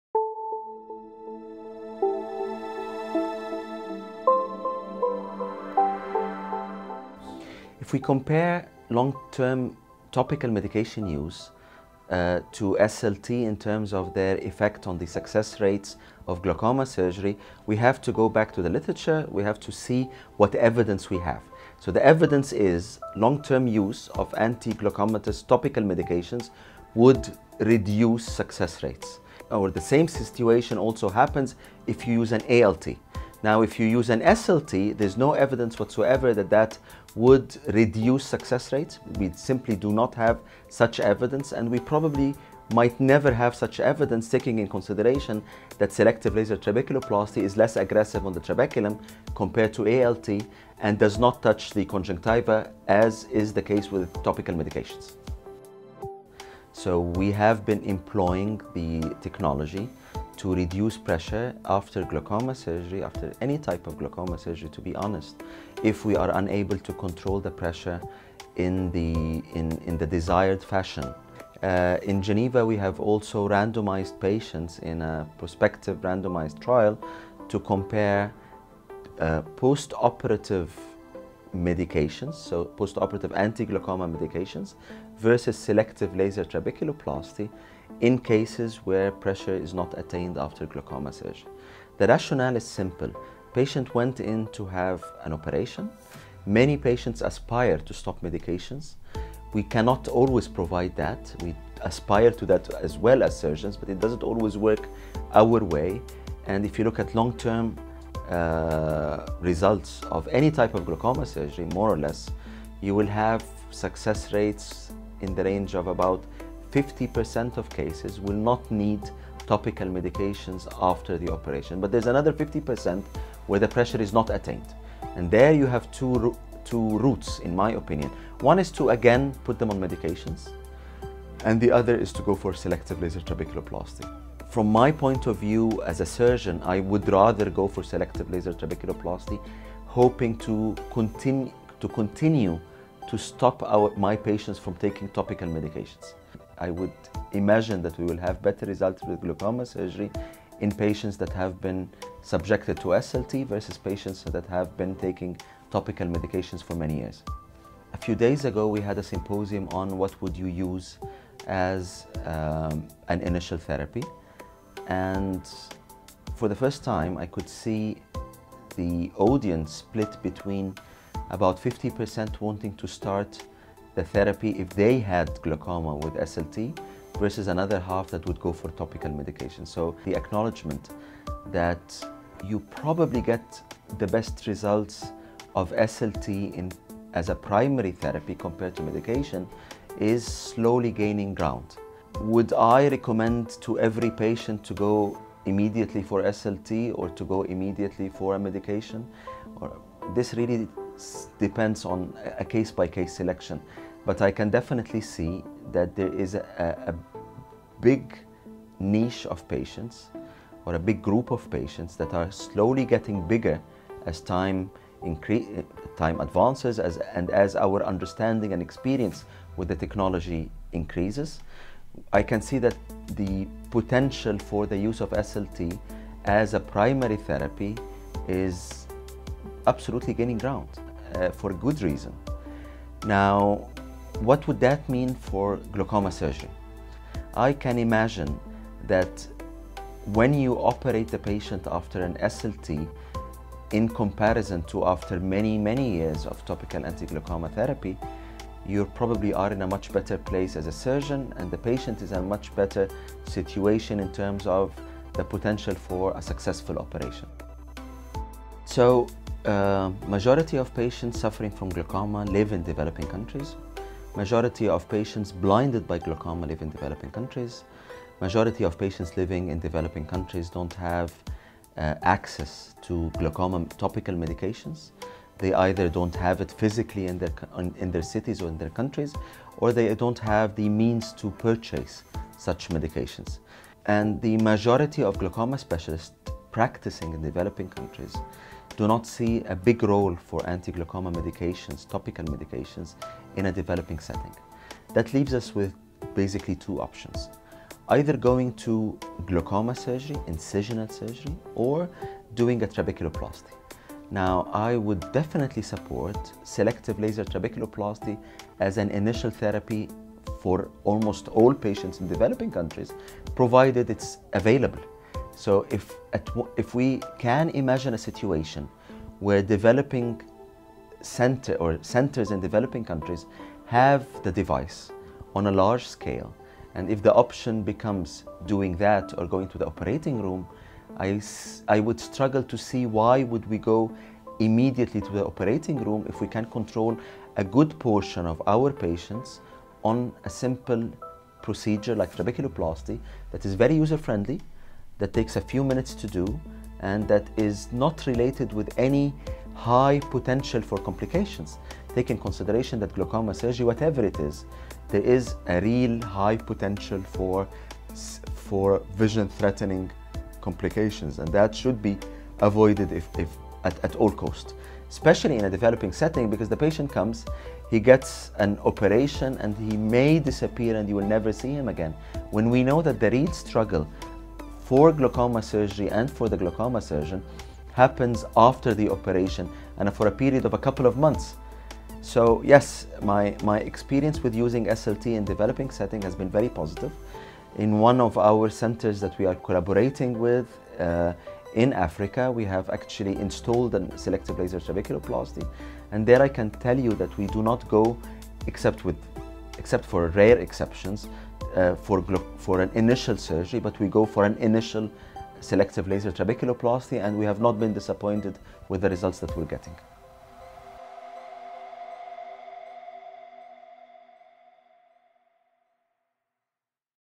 SLT and the Glaucoma Surgeon -- Video Interview with Tarek Shaarawy, MD, Geneva, Switzerland
SLT and the  Glaucoma Surgeon

Video Interview with Tarek Shaarawy, MD, Geneva, Switzerland

Not only can SLT achieve sustained IOP reduction for the majority of your glaucoma patients, but it has also been hypothesized to improve the prognosis of glaucoma surgery as compared to long-term medication use.  Watch the video interview with Dr. Shaarawy to learn more.